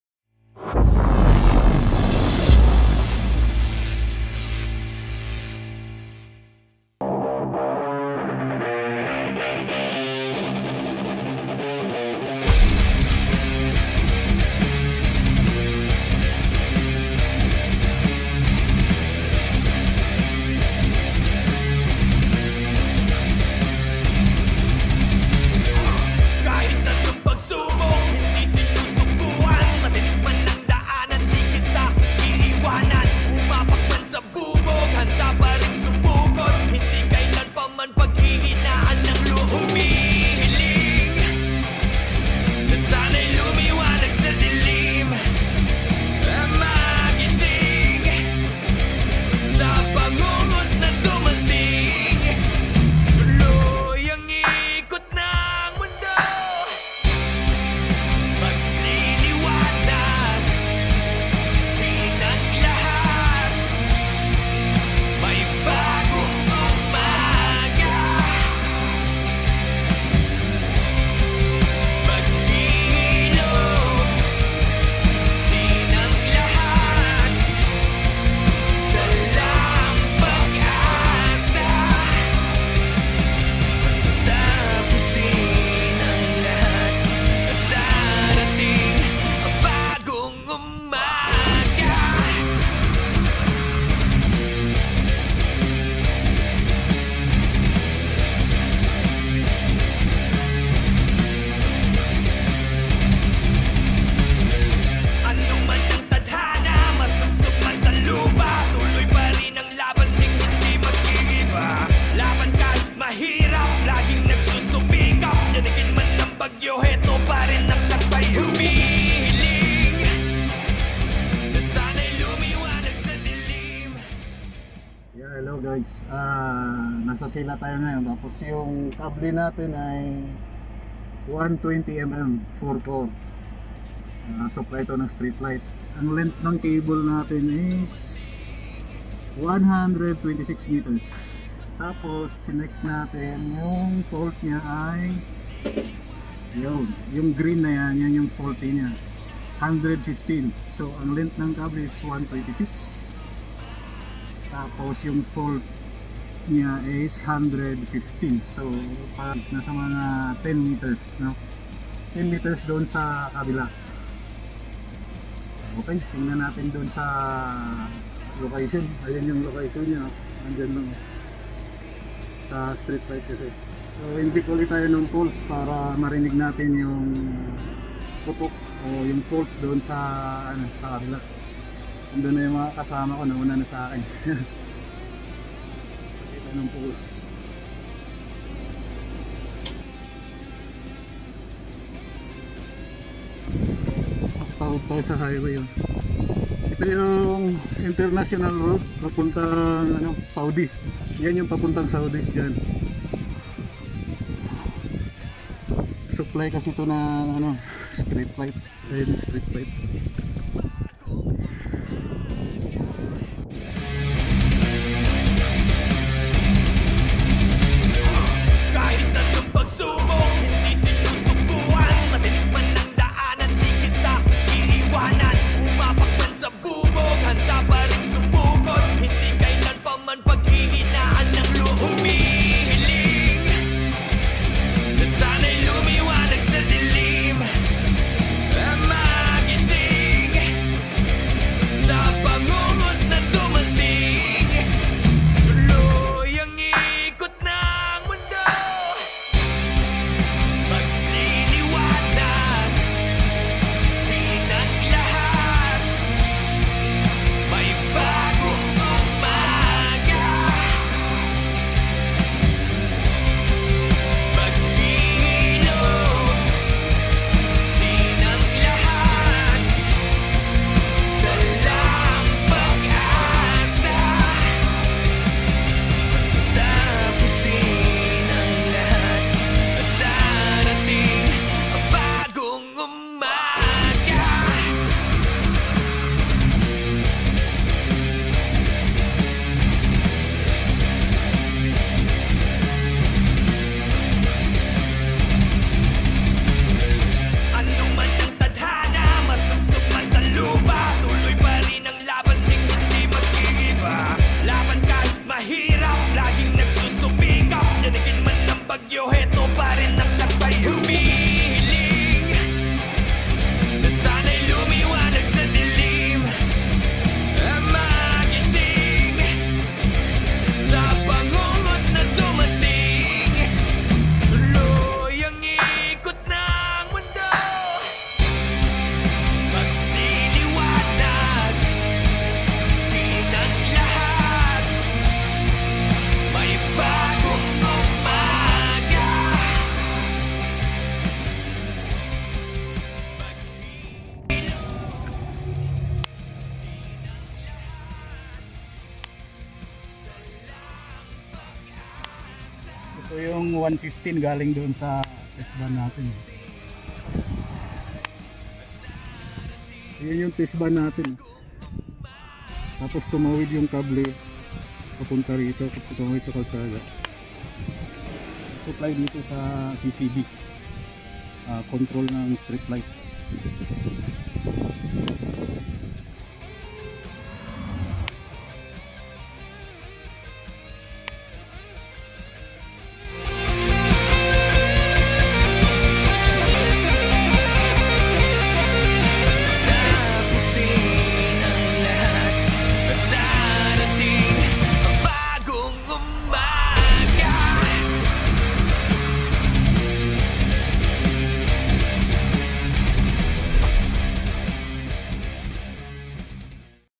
Emergency Work Sila May 10,2020
Underground cable fault at SILA western part of ABU DHABI this is located near SAUDI ARABIA border.